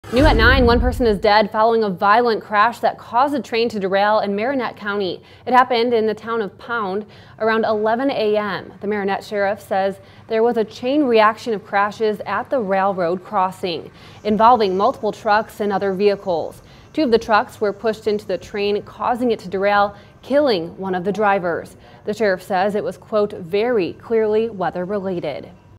Chain-reaction crash in Wisconsin fog kills 1, derails train
A chain reaction of vehicle collisions in foggy weather at a railroad crossing in northeastern Wisconsin killed one of the drivers and caused a train to derail.